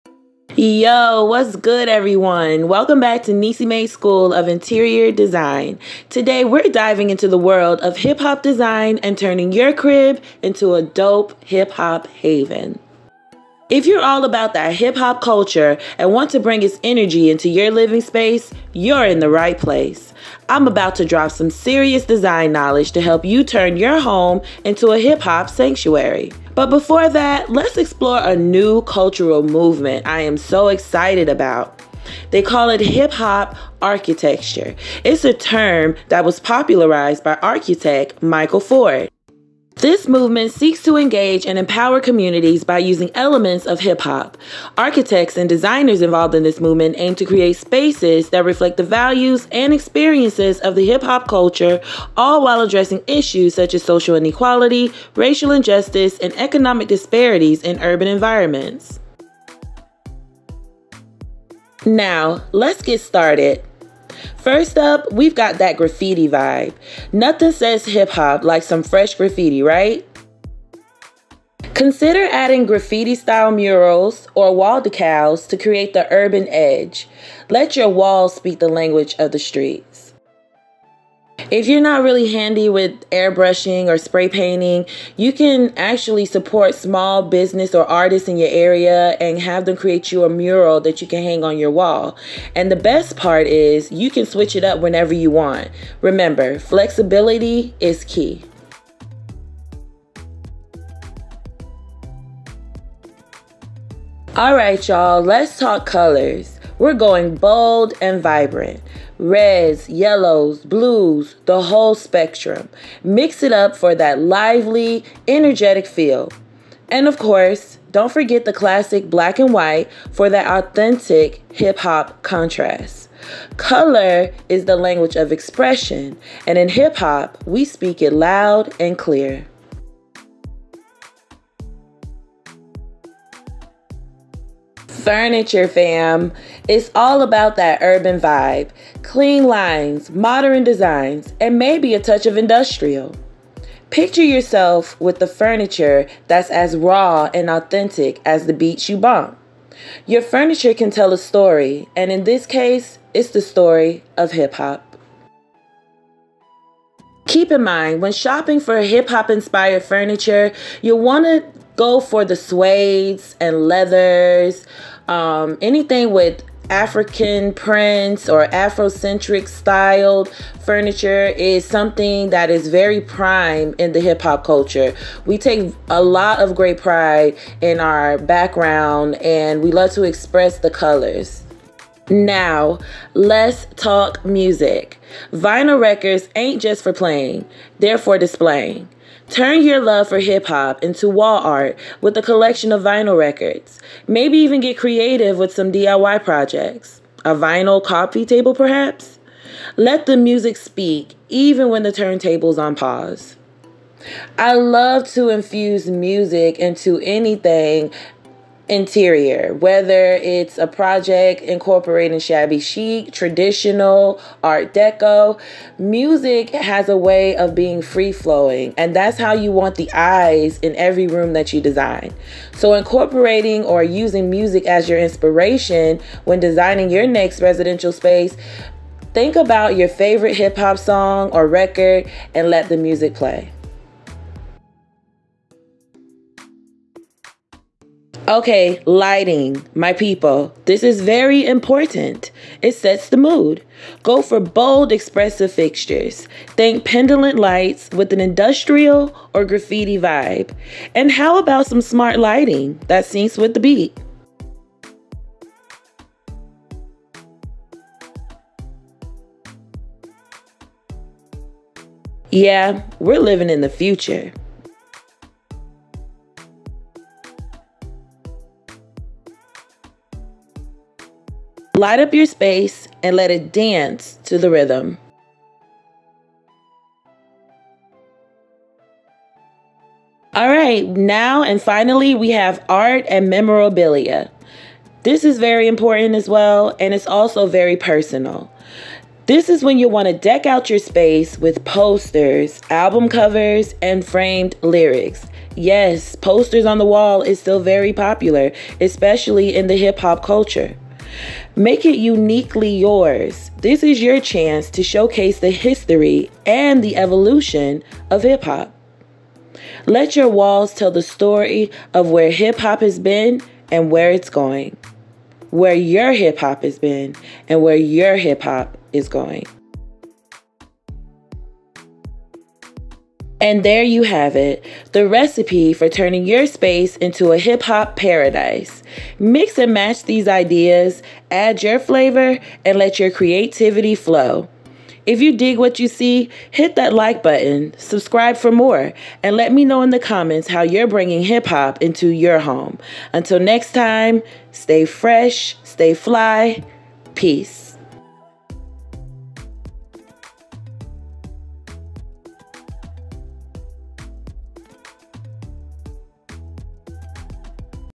6 WAYS TO TURN YOUR SPACE INTO A HIP HOP PARADISE‼️🤴🏾🎚🎛🎤🎧🎵🎶HIPHOPDESIGNINSPIRATION
If you're all about that hip hop culture and want to bring its energy into your living space, you're in the right place. I'm about to
drop some serious design knowledge to help you turn your home into a hip hop sanctuary.